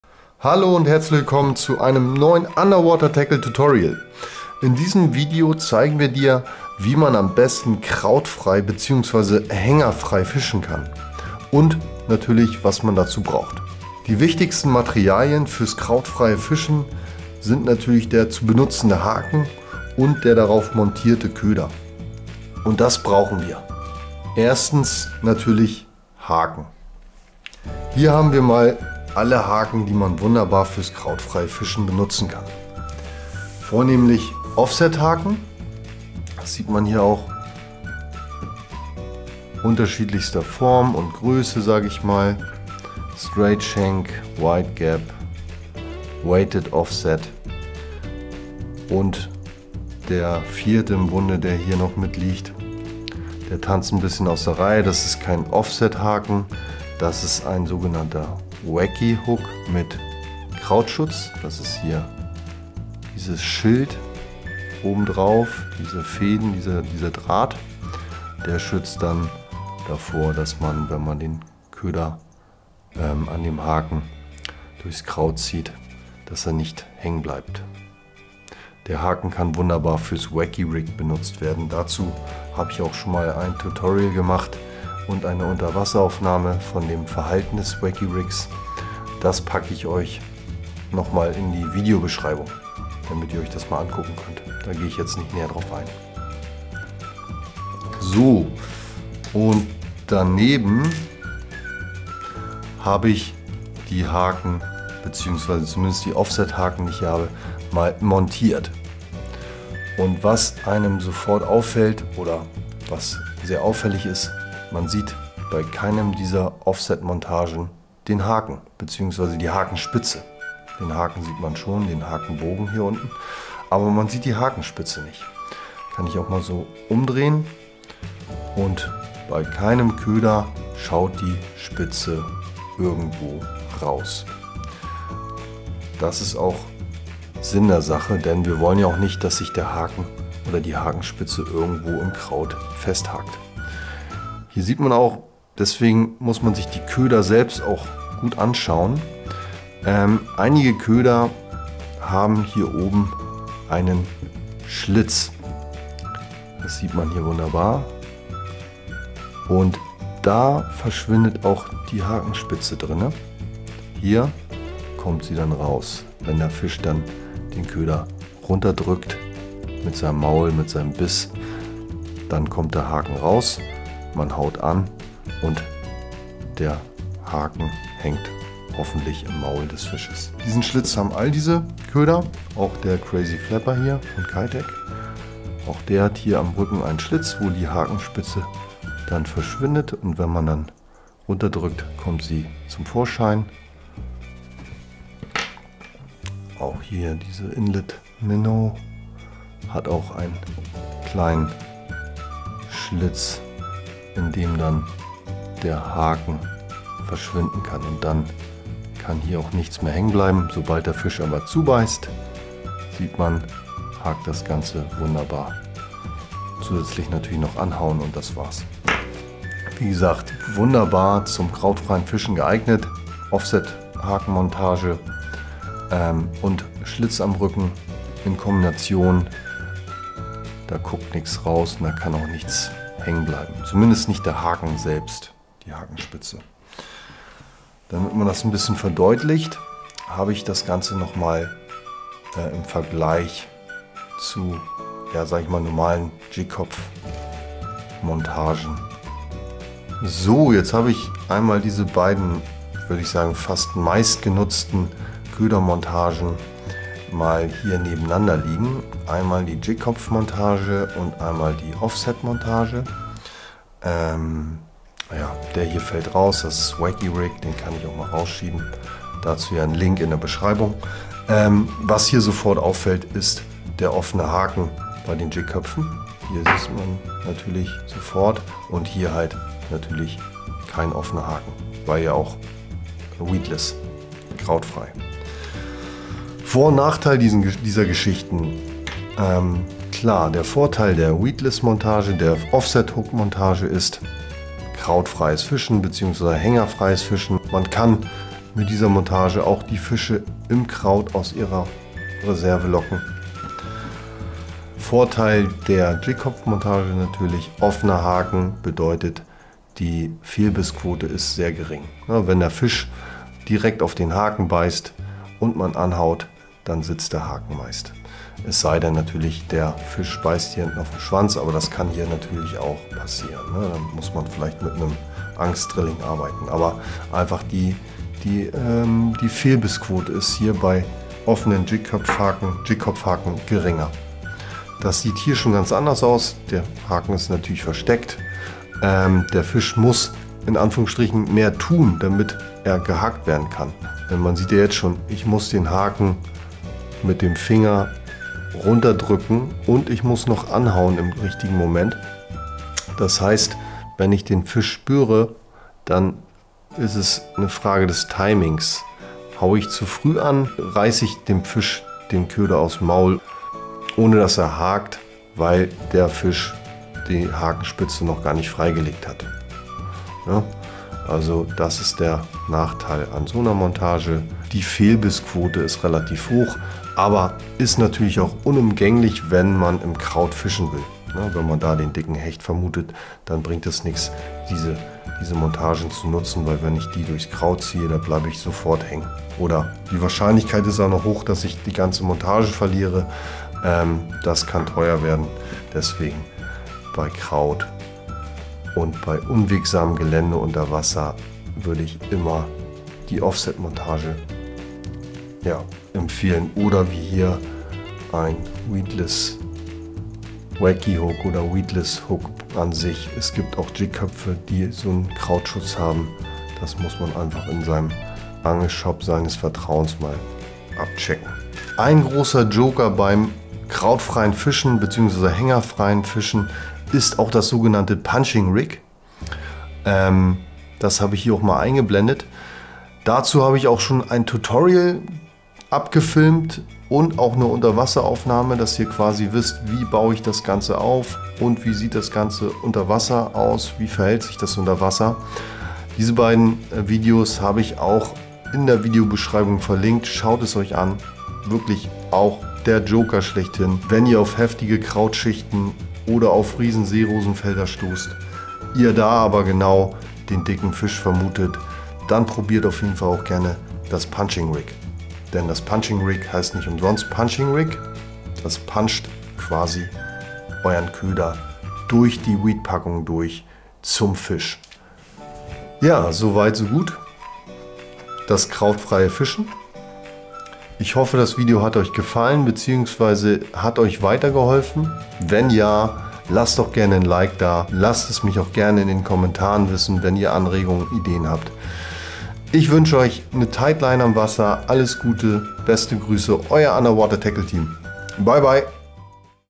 Krautfrei angeln! Wie vermeide ich Hänger und Abrisse beim Spinnfischen.
In diesem Video zeige ich Dir, wie Du am besten kraut- bzw. hängerfrei auf Raubfische angeln kannst. Das wird Dir helfen, auch unwegsame Gewässer erfolgreich befischen zu können.

Das wurde im Video zum krautfreien Fischen benutzt:

Die Offset-Haken:
- Decoy S-Switcher Worm102 - Gr. 3/0 (1.0g)
- Decoy 220 Cover Finesse Hd Worm hook - size 2/0
- Gamakatsu Offset Worm hook EWG - size 2/0
- Gamakatsu Worm 34 Offset Wormhook - size 2/0

Die Köder:
- Keitech - Crazy Flapper
- Nories - Inlet Minnow
- Owner - Nervous Rex

Was haltet Ihr von diesem Format? Wollt Ihr mehr davon sehen? Helfen Euch diese Videos?